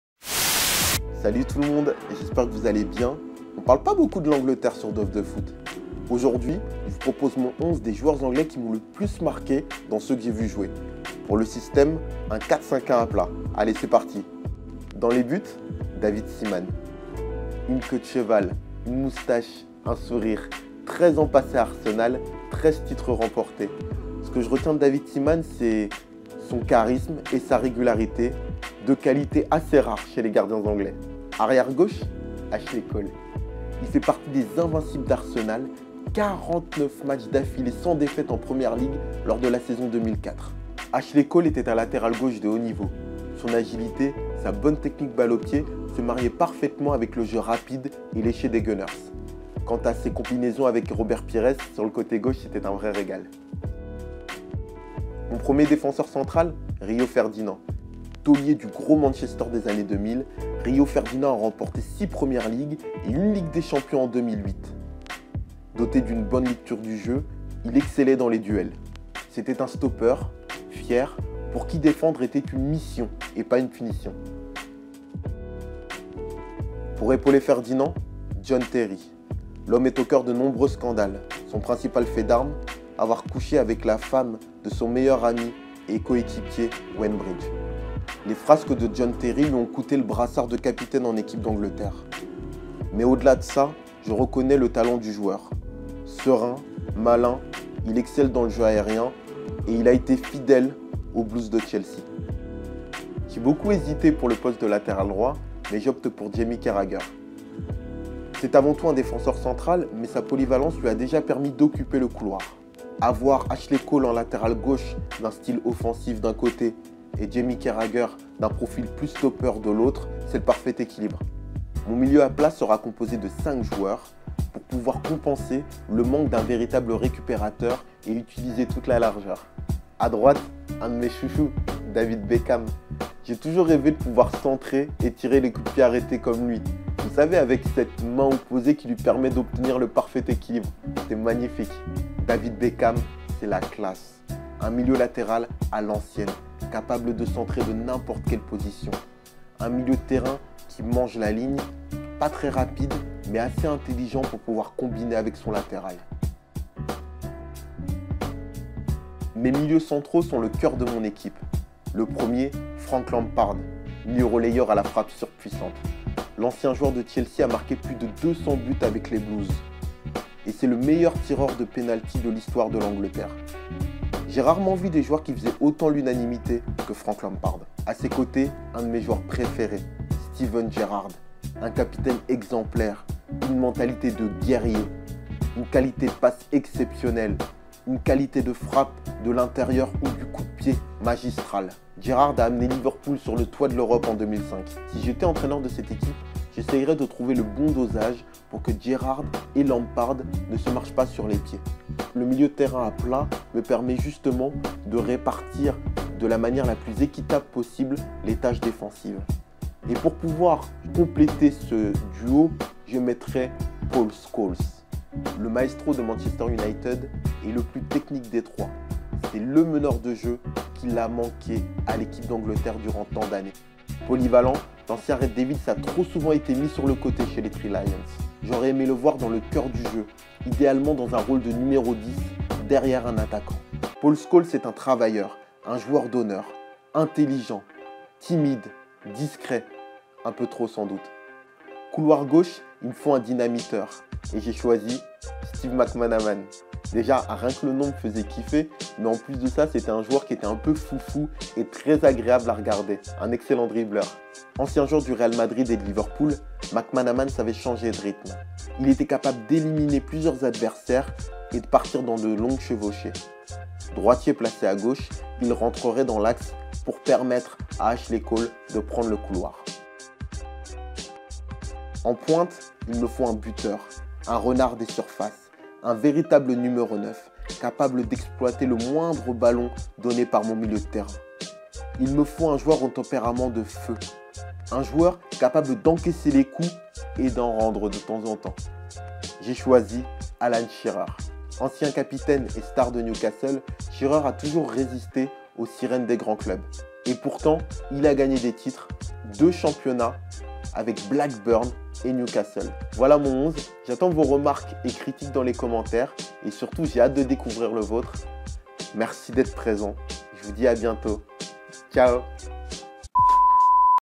MON 11 DE LÉGENDE DE L'ANGLETERRE
Salut tout le monde, voici mon 11 de légende anglais. Une équipe composée de joueurs que j'ai eu l'occasion de voir évoluer...